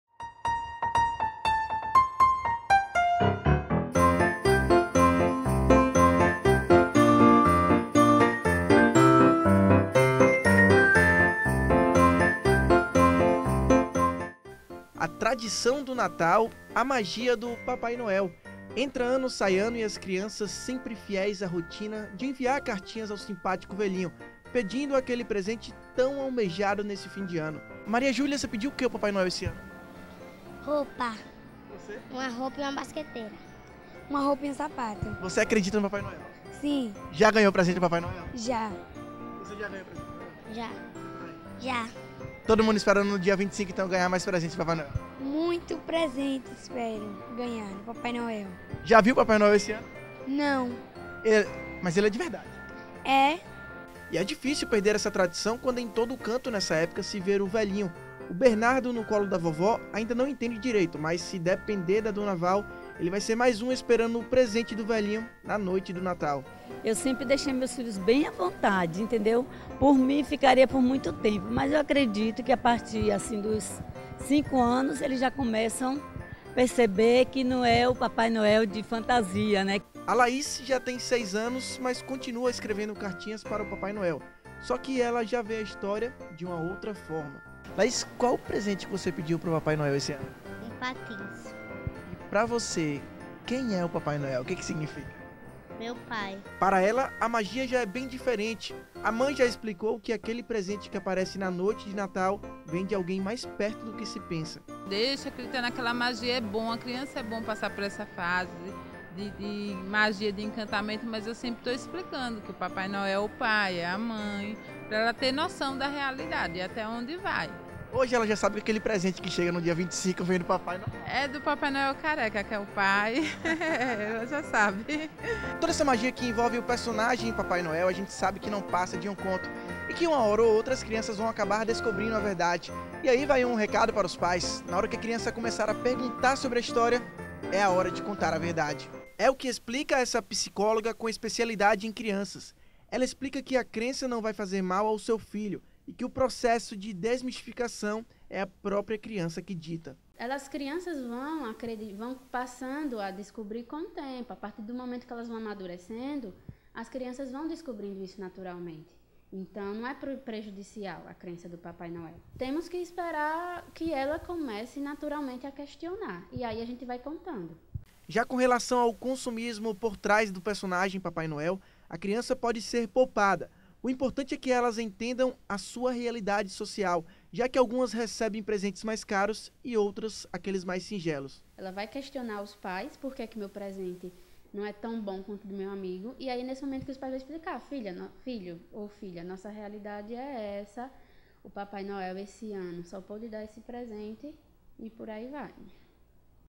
Até que ponto crença no Papai Noel pode ser boa para criança
A maioria das pessoas em algum momento da vida acreditou na magia que envolve o personagem do Papai Noel. Aquela estória que diz que velhinho atende milhões de pedidos de crianças por todo o mundo e, na noite de Natal, vai deixar o presente pela chaminé da sua casa. O fim da fantasia pode vir de diversas formas. Nossa reportagem conversou com uma psicóloga infantil para entender esse processo.